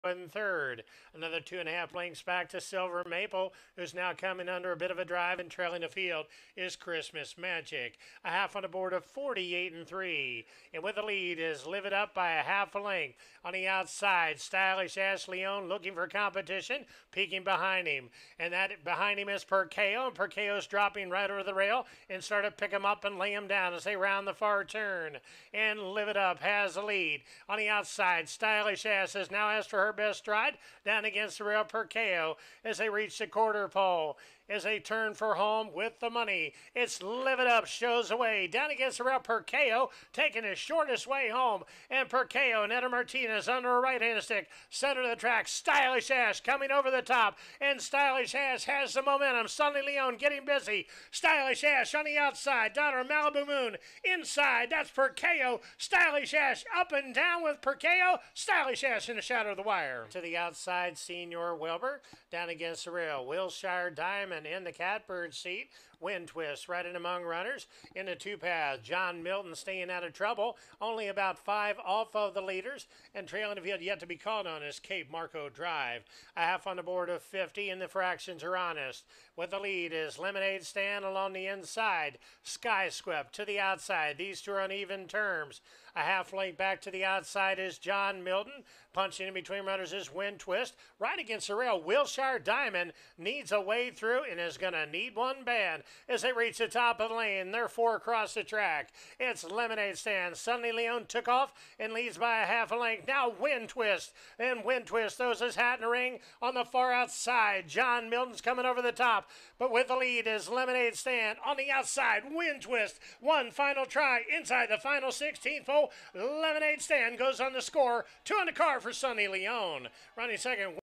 4ª. y 5ª. Carrera - STYLISH ASH y LEMONADE STAND con Sonny León en Belterra Park
DOS EN CASA
Así ganó "El Magnífico" Sonny León la 4ª. y 5ª. Carrera de hoy miércoles 13/07 en Belterra Park con los ejemplares 1A STYLISH ASH apretando en los finales para ganar por medio largo y luego con el #4 LEMONADE Stand crecido en punta.

4ª. Carrera
Lote: 1 Mile 70 Yards | Fillies and Mares | 3 Year Olds And Up | CLAIMING : $7,500 | PURSE: $13,400

Pizarra:
1AStylish AshJ: S. Leon 121Lbs3.202.10
5Live It UpJ: I. Ayala 123Lbs 2.40
1Silver MapleJ: J. McKee 121Lbs
Times in 5ths: :241   :483   1:141   1:412   1:454
5ª. Carrera
Lote: 1 Mile | Open | 3 Year Olds And Up | CLAIMING : $5,000 | PURSE: $9,500

Pizarra:
4Lemonade StandJ: S. Leon 121Lbs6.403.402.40
2Wind TwistJ: J. McKee 121Lbs 2.602.10
5John MiltonJ: J. Bracho 121Lbs  3.00
Times in 5ths: :253   :50   1:15   1:28   1:412